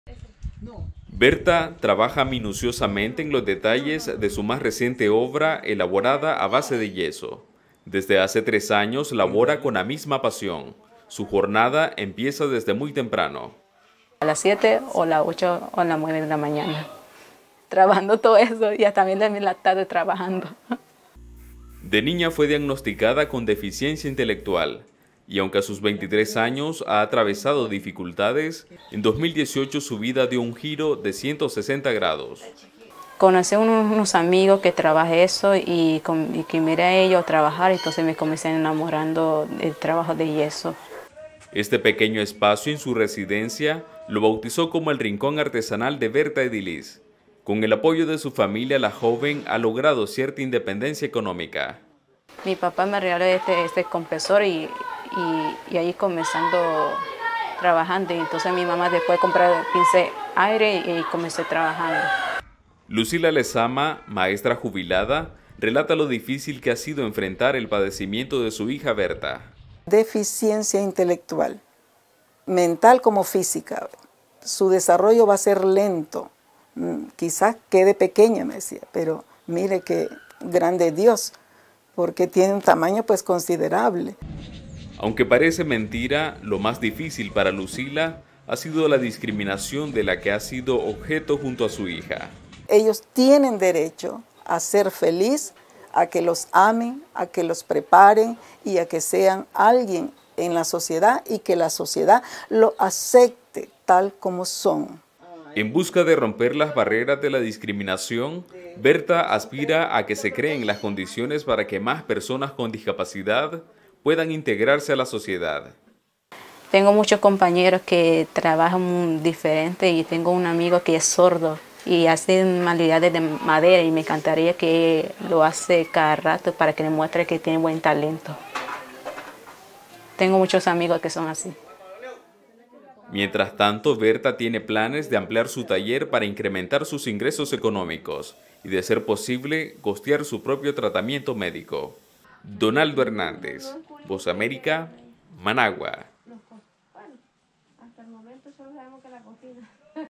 Joven con discapacidad convierte el barro y el yeso en obras de arte en Nicaragua
En Nicaragua, una joven con discapacidad intelectual se convirtió en artesana, lo que le ha permitido lograr independencia económica. Sus piezas, que elaboradas a base de yeso y barro, son compradas por nicaragüenses que admiran su talento. Donaldo Hernández con su historia.